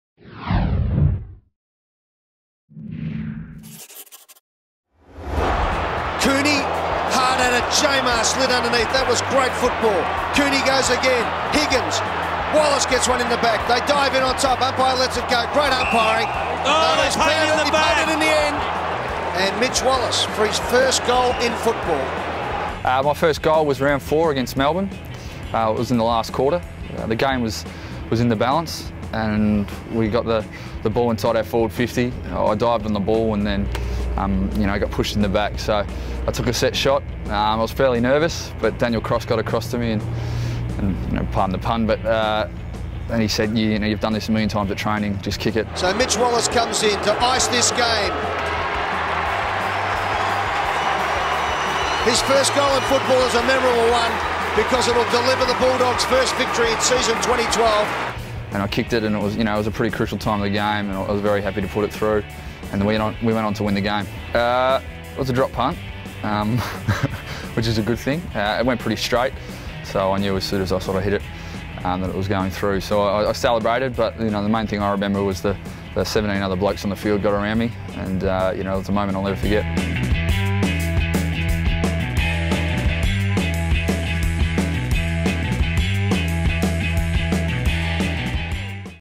My First Goal - Mitch Wallis
Each week a Bulldogs player will recount their memories of their first goal in the AFL. This week Mitch Wallis looks back at his first ever goal against Melbourne in 2012.